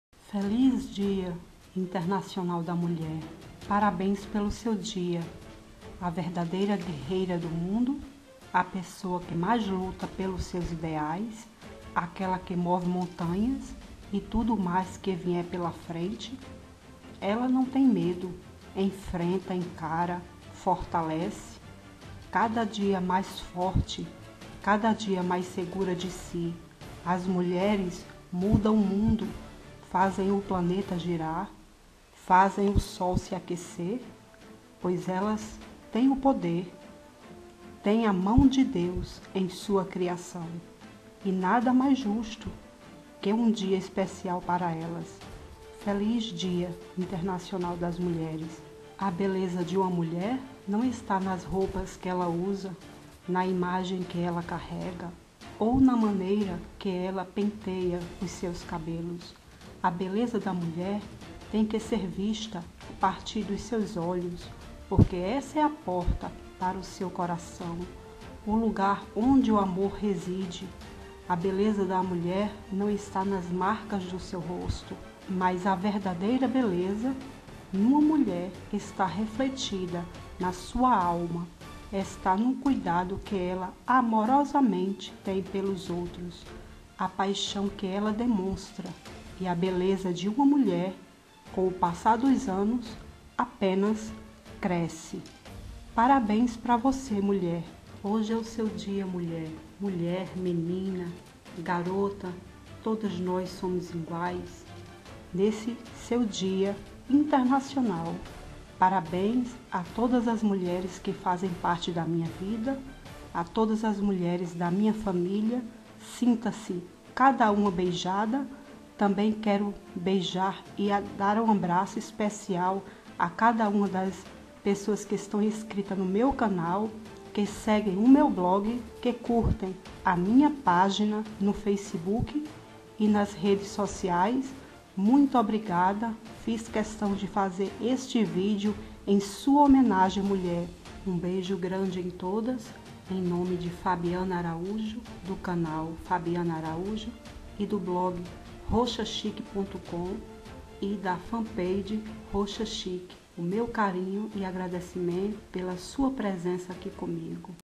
Feliz Dia Internacional da Mulher!
Assistam em HD para melhor Imagem e Som. Hoje vim homenagear a todas as mulheres, meninas que fazem parte da minha vida. Não Pude deixar passar em branco este dia. Sinta-se abraçada, cada uma. Com Carinho! Fabbiana Araújo :-)